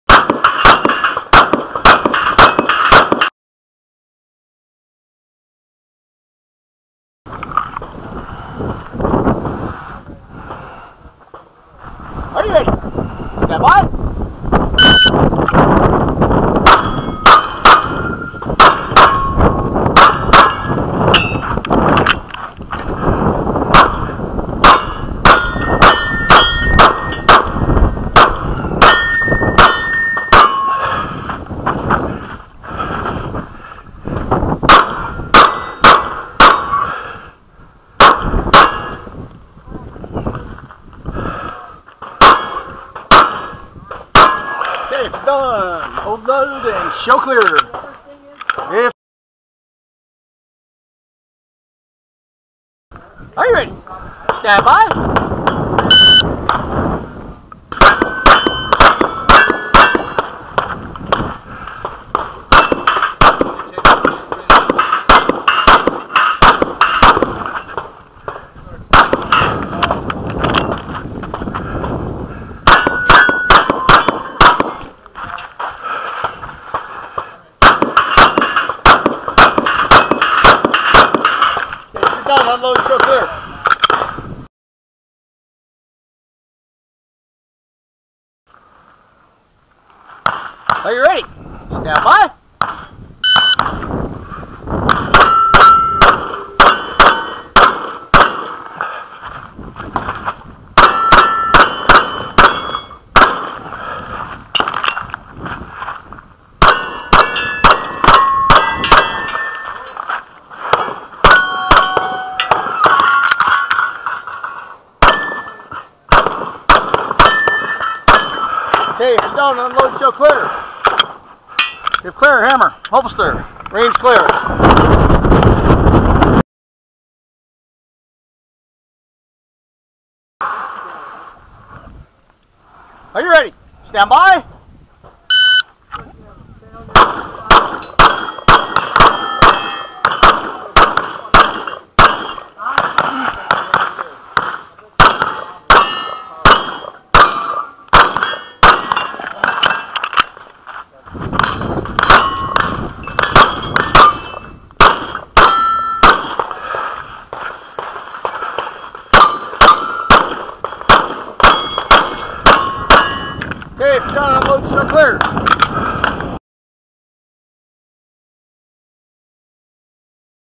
7-19-22 Rio Salado Tuesday Night Steel, Mesa, AZ
Hot, hot , hot… like 106 degrees hot and sunny. But that’s Arizona in July! Just came back from a 7 week road trip and it feels good to have a gun in my hand and hearing the timer beep! The heat and no shooting for 7 weeks shows in the times.  None the less it is good to be back.